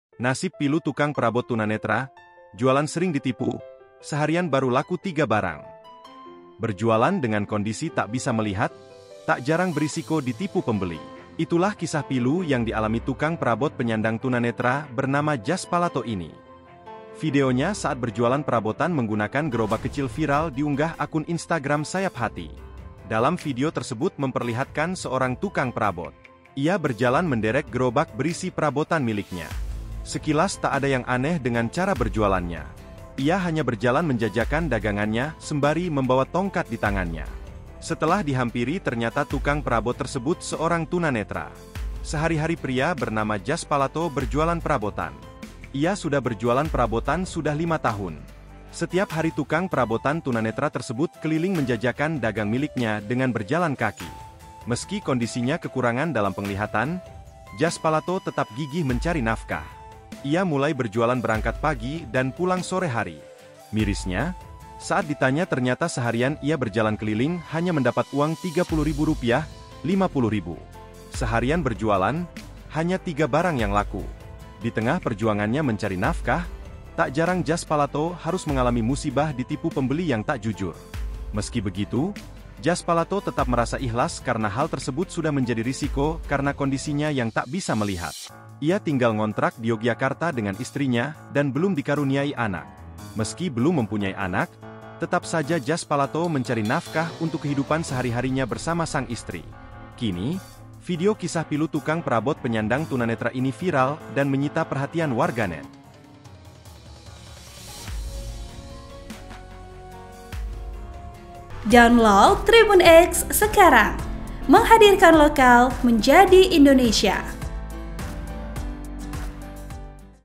Nasib Pilu Tukang Perabot Tunanetra, Jualan Sering Ditipu, Seharian Baru Laku 3 Barang
Berjualan dengan kondisi tak bisa melihat, tak jarang berisiko ditipu pembeli.

Itulah kisah pilu yang dialami tukang perabot penyandang Tunanetra bernama Jaspalato ini.

Videonya saat berjualan perabotan menggunakan gerobak kecil viral diunggah akun Instagram @sayaphati.

Program:
Host:
Editor Video: Mayang Putri Anugraha
Uploader: